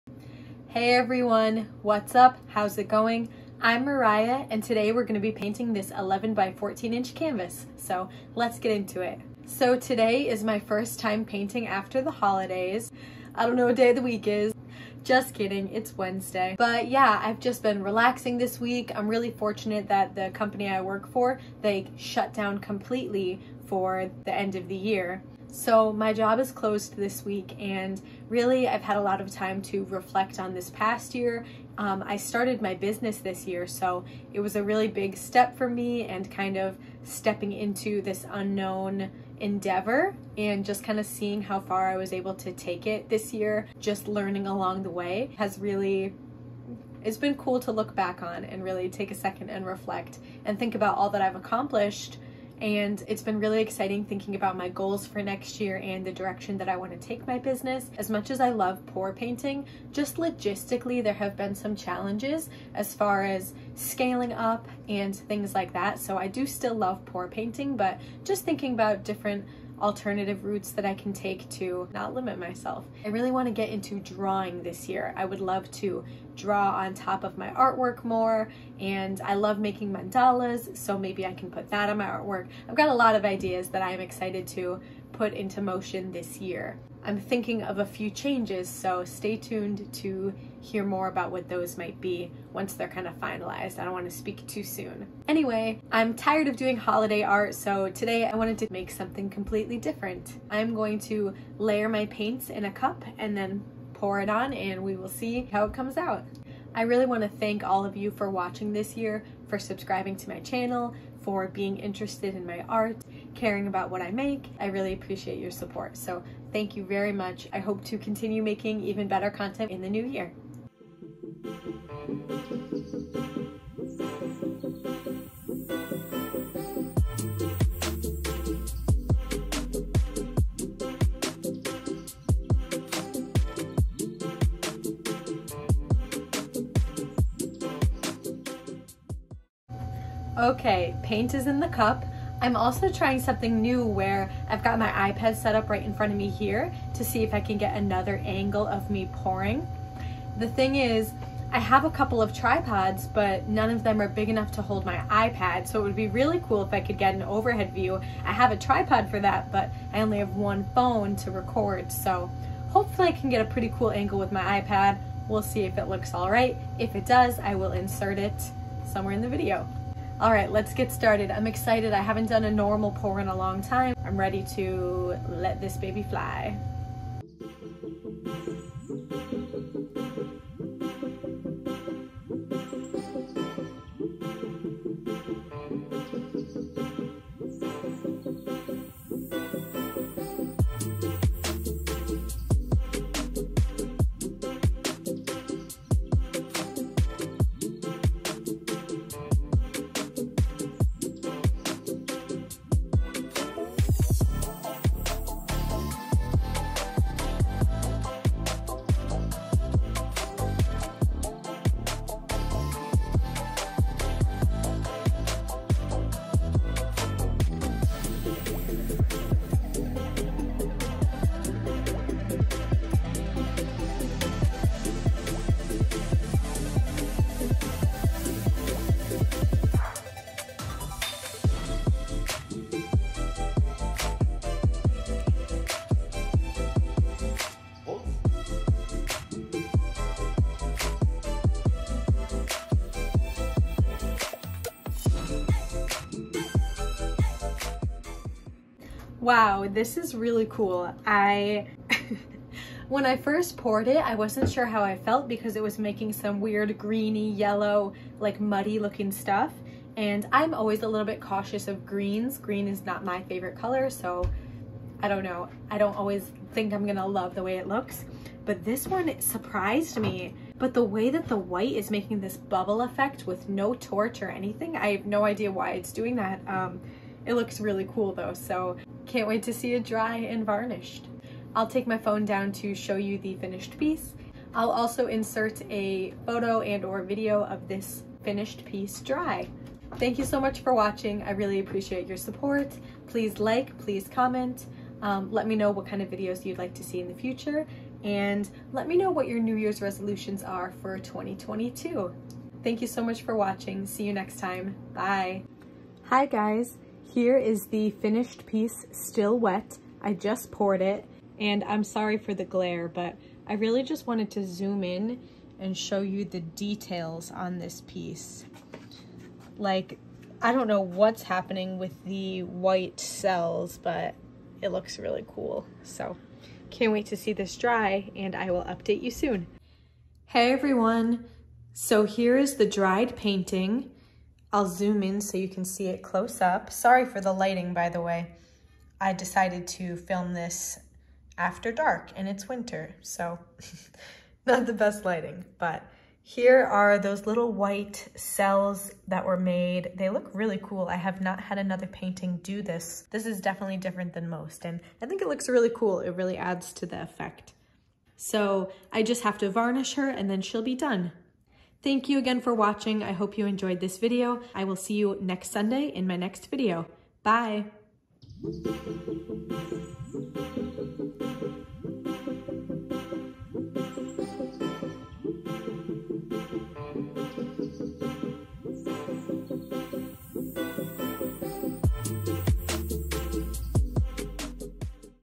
New Year, New Pour Paintings! WELCOME 2022 Fluid Art, Acrylic Pour
🎉💖 I can't believe how quickly 2021 went by, but it was a huge year for me - the year I started Daydream Drips LLC! I'm finally settling into a routine after many changes last year (with maybe more to come), and I'm so excited for the new art to be made in 2022! The only reason I'm glad the holidays are over now is I can go back to making colorful art ☺️ I was getting a little tired of the Christmas theme so today I decided to throw a bunch of colors in a cup and see what I got! 🤷🏼‍♀️ I also set up a new camera to get a different view of the pour process. Let me know which perspective you like more in the comments! ✨